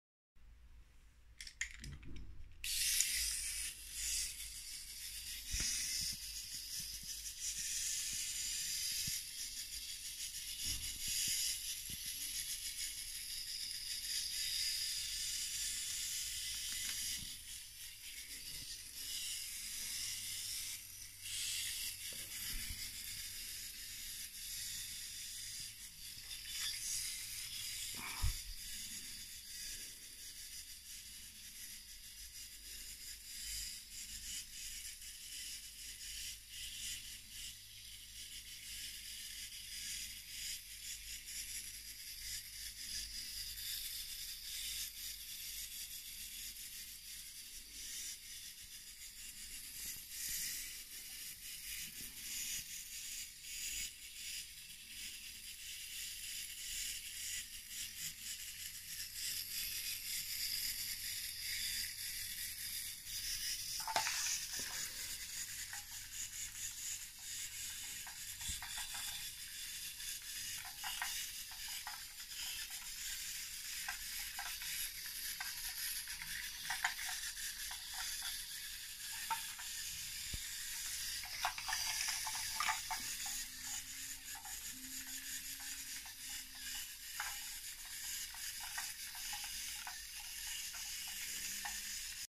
"Magic Pen Toy" and LEGO (2)
The toy motor seems strong enough to move a light LEGO car. But the magnetic sheet isn't good enough to guide it.